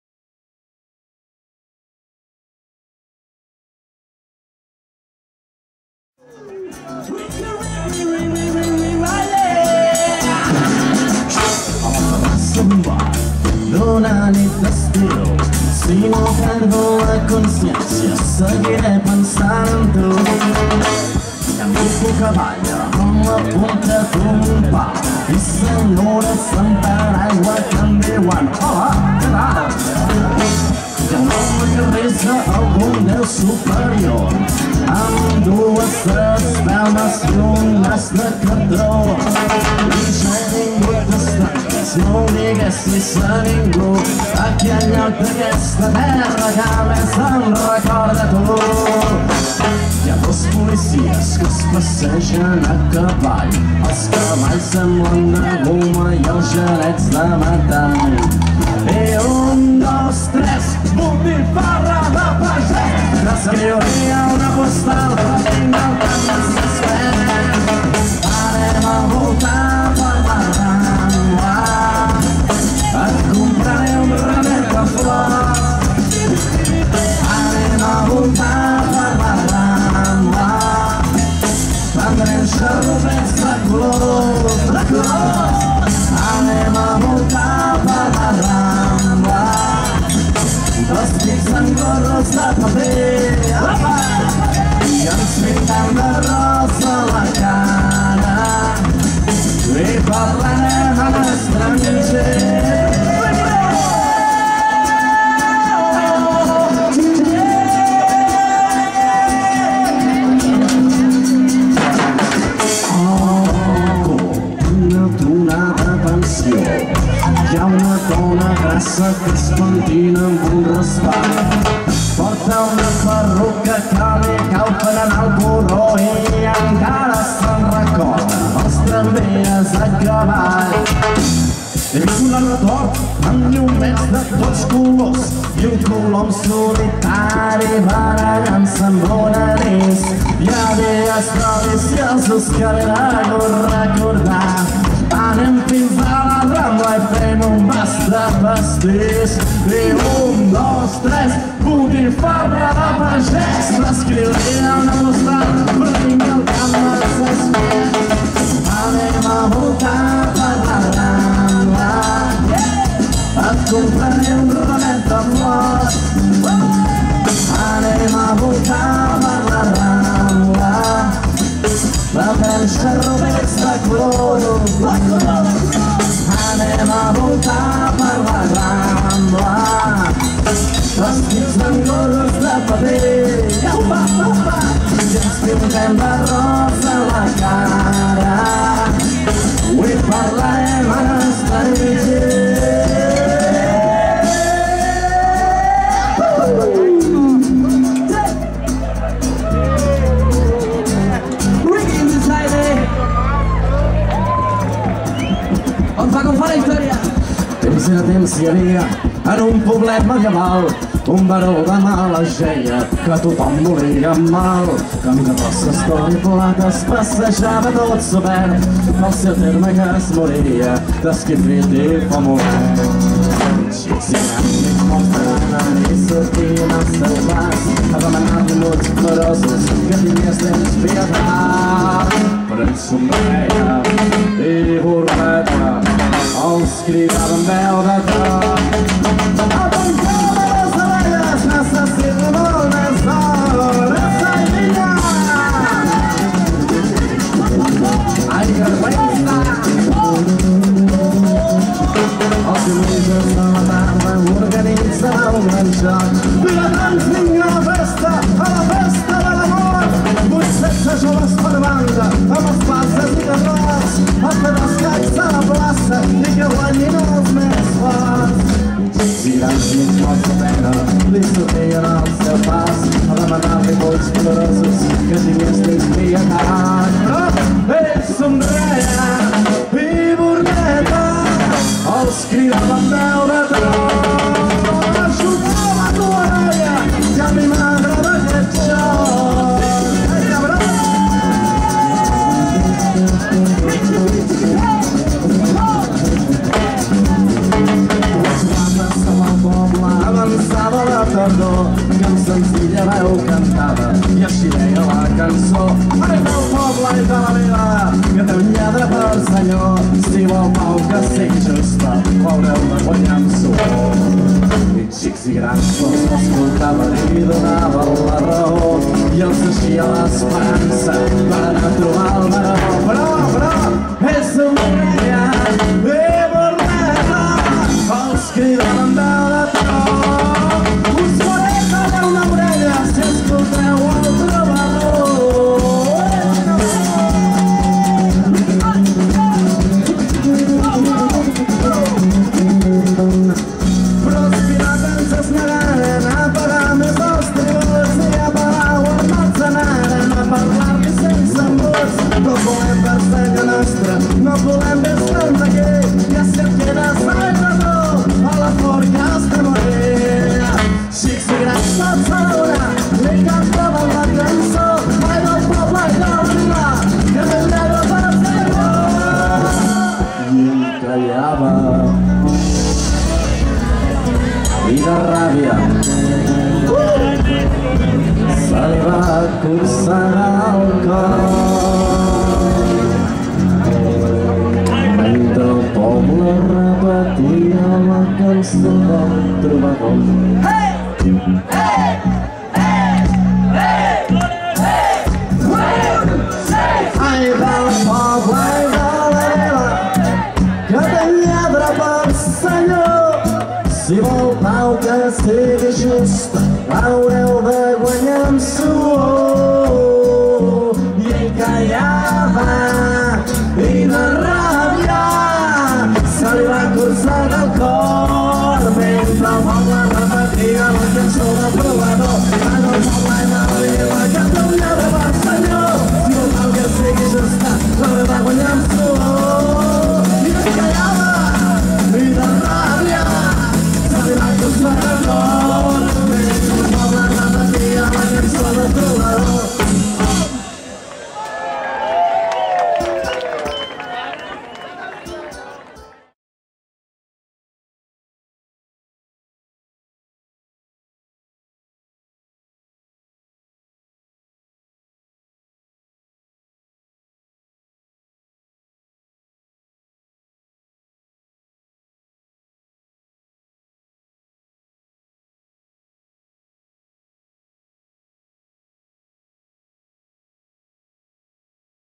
Rumb al Bar (2/3) : Live @ Manani Rock FM 2010
Rumb al Bar, performing at Manani Rock FM 2010, Vilanova i la Geltru, 30/07/2010.